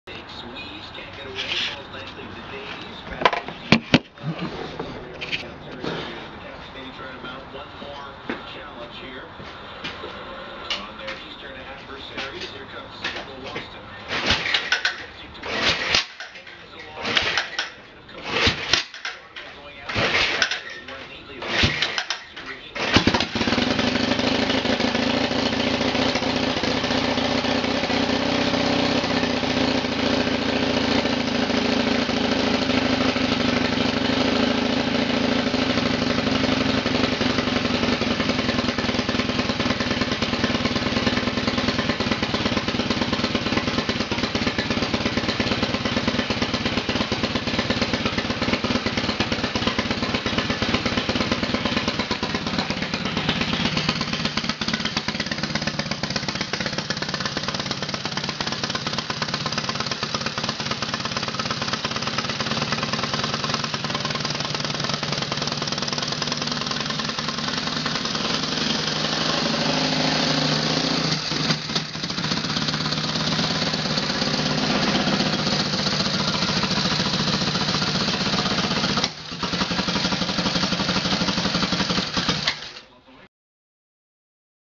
25 Harley JD
1925 Harley JDCB first time running in decades.